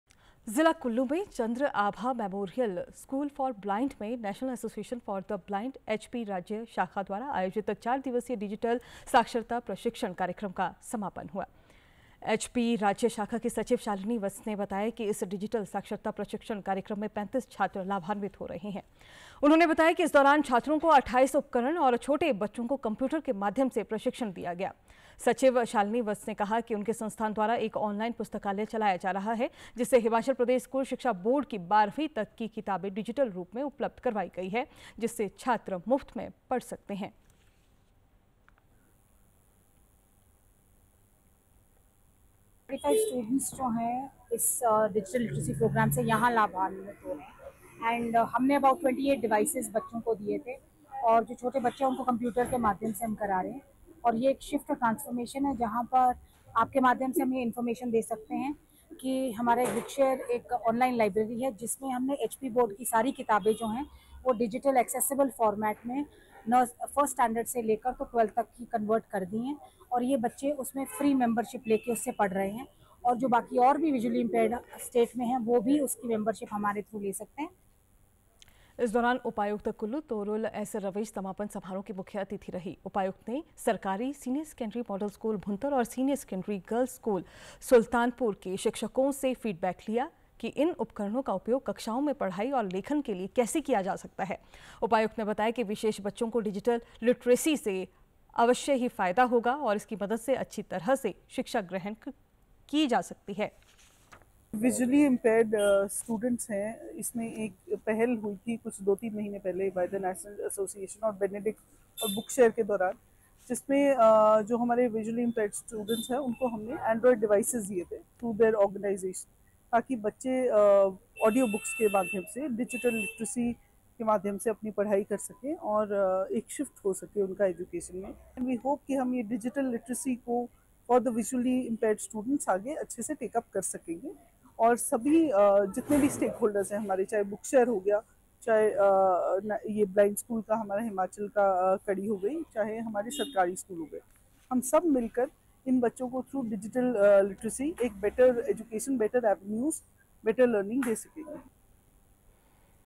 कुल्लू: डिजिटल साक्षरता प्रशिक्षण कार्यक्रम हुआ संपन्न
कुल्लू: डिजिटल साक्षरता प्रशिक्षण कार्यक्रम हुआ संपन्न - प्रशिक्षण कार्यक्रम से 35 छात्र हुए लाभान्वित।